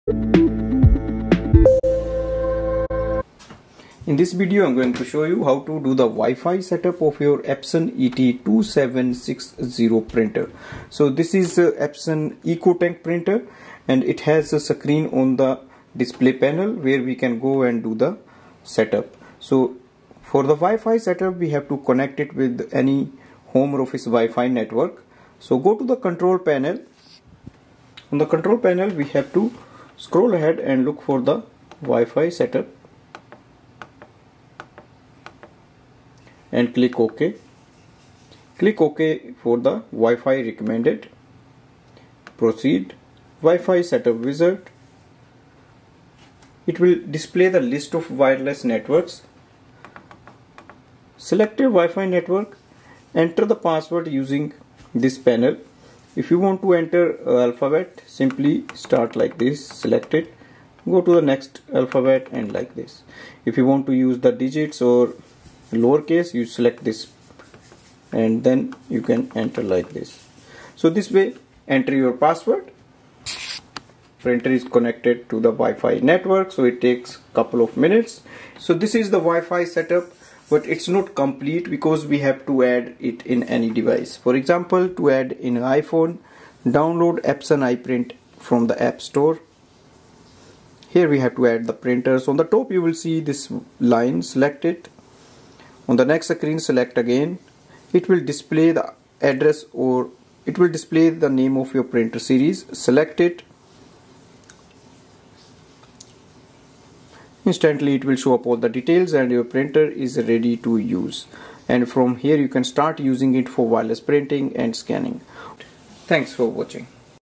Epson Ecotank ET-2760 WiFi Setup.
This video reviews the method to do the complete WiFi setup of your new Epson Eco Tank ET-2760 All-in-one Printer. This video shows how to connect to a home wireless network, for wireless Scanning and printing.